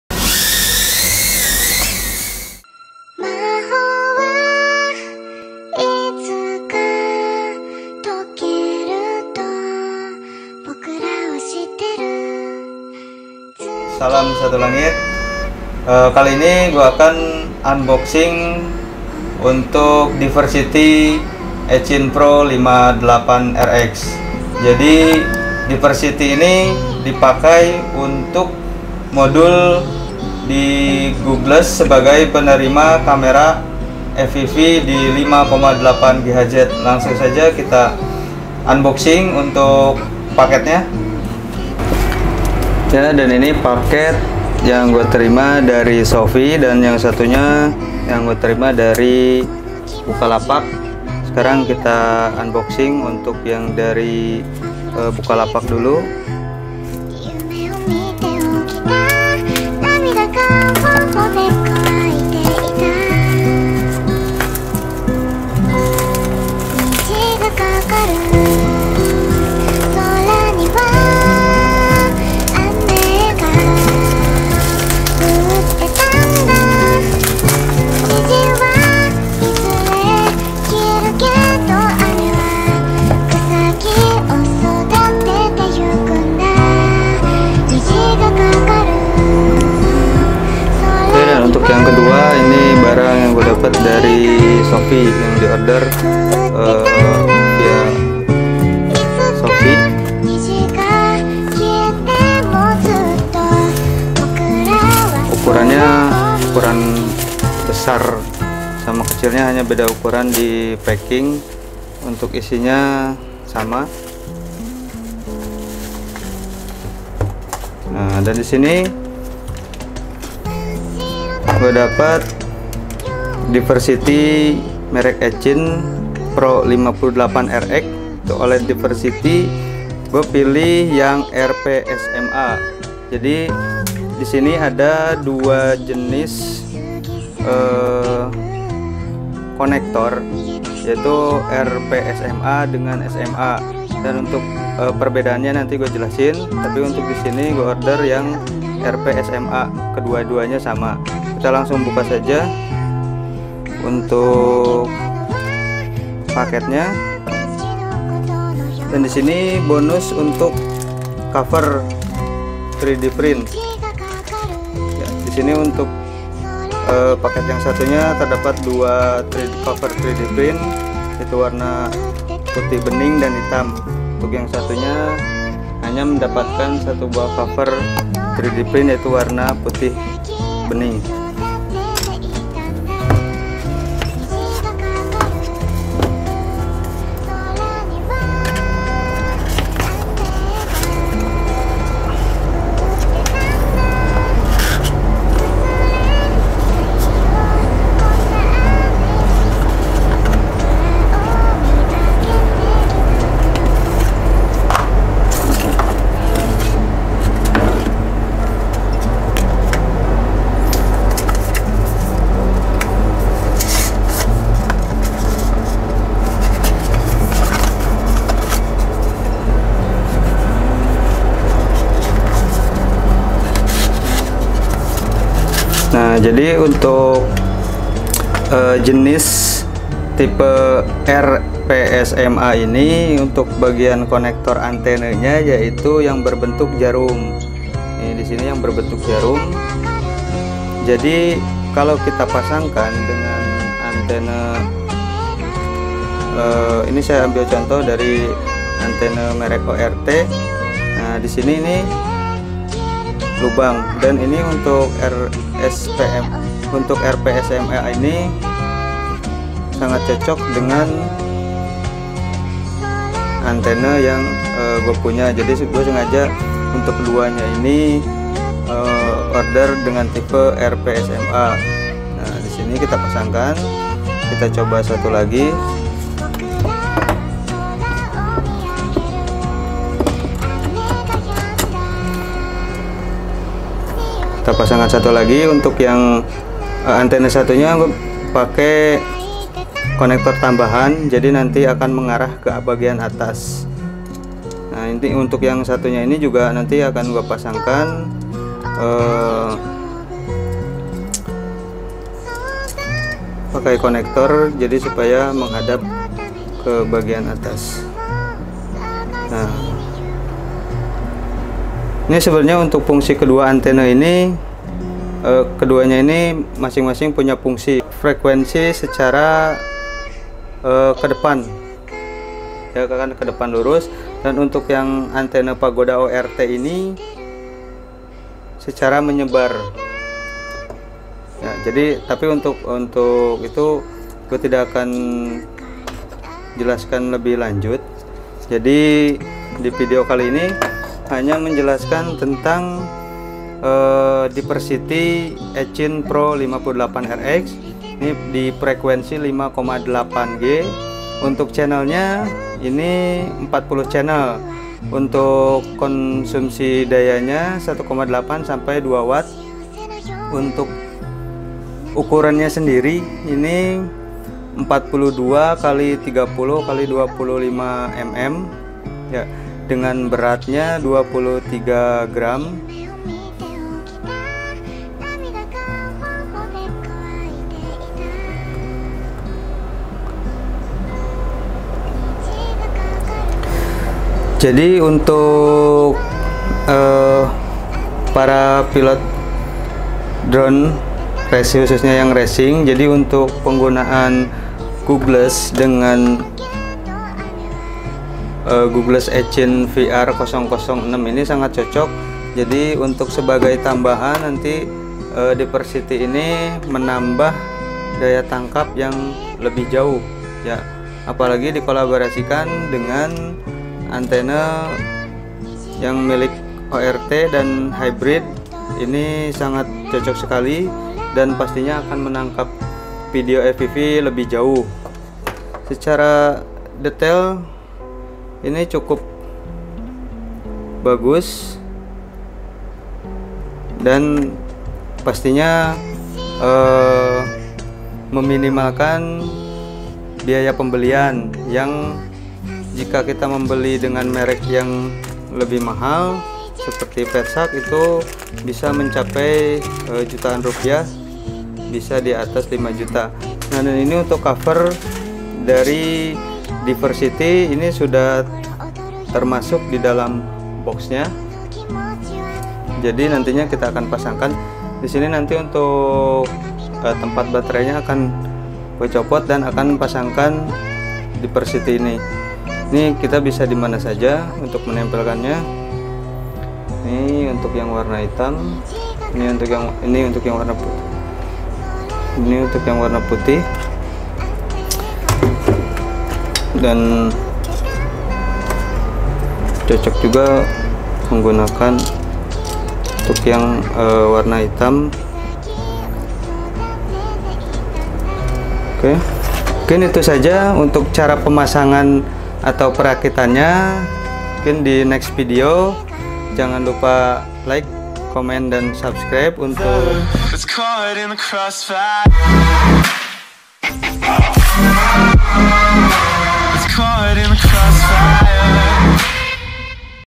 DIVERSITY EACHINE 58 PRO
Deskripsi Eachine Pro58 OLED Modul Diversity FPV Receiver 5.8G 40CH - SMA
Description :

Item Name : PRO58 RX

Frequency : 5.8G

Channel : 40CH

Size : 42 x 30 x 25mm (check detail dimension on picture)

Weight : 23g

Specifications:

Duo way working signal/Diversity;

All frequencies scanning, and result selectable;

Easy and fast way to show wireless information;

Great RX part for FatShark or similar Goggles;

40ch included RaceBand;

Nice sensitiveness -92dBi;

5V working, power from goggles;

partially control by goggles buttons, the ch+ and ch-;

SMA versions

1built-in beeper to indicate your operations

Features :

1. Directly control by the buttons (ch+ and ch-) on FatShark Goggles, but limited with channels selection 1-8, due to FatShark inside S/W.

2. Customized booting LOGO of EACHINE.

3. Searching mode show you all frequencies working and their strength, and you can select them on the result table. We present all frequencies in order of A-B-E-F-R sequent, each channel has a small bar to show its strength, the longer bar stand for stronger signal.

A typical searching result shows: there are 6 signals, and E1(5705M) is most strong. You can press confirm to select it, or you can up or down to select another channel and press confirm to show it,

4. Main working interface offer abundant information, including: working frequency name (like E1, in bigger size letter), exact working frequency (like 5705M), working mode (like AUTO, working on RX A, Auto way), and signal strength (right-up Conner), and if you work on diversity mode, there will be current working RX (A or B) just before the strength sign.

5. Simple and easy interface way to show you most useful information.

6. Easy to use up-down-select 3 ways button

7. Compatible with Fatshark Telepoter V3, Dominator V2 & V3 & SE and Fatshark HD V1, V2 & V3 goggles
8. Great 5.8G performance

Beli Diversity Eachine PRO58 RX 40CH 5.8G OLED VRX FPV Receiver